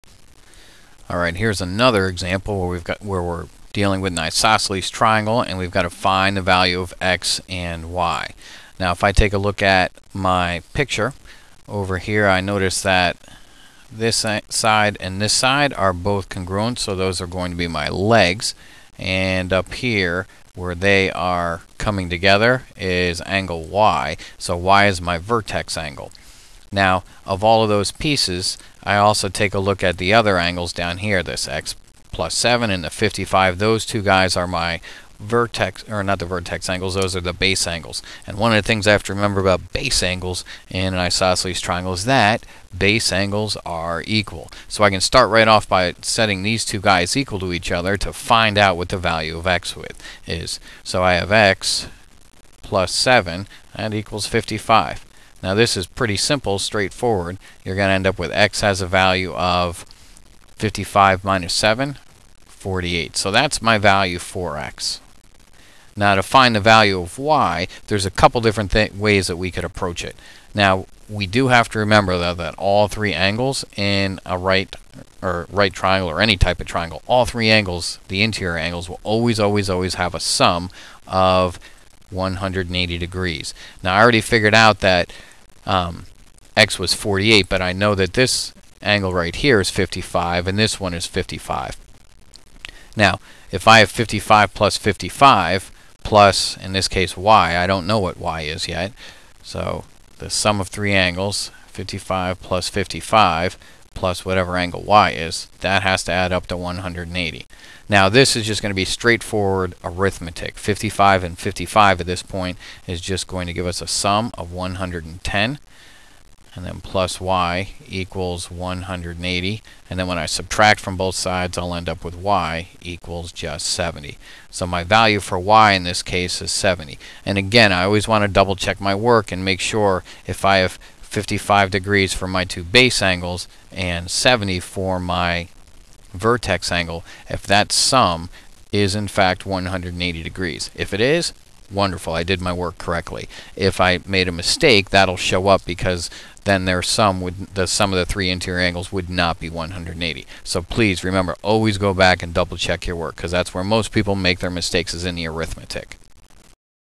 4-6 & 4-7 Finding Angle Measures in Isosceles Triangles 2
Finding the angle measures given the base angle in an isosceles triangle.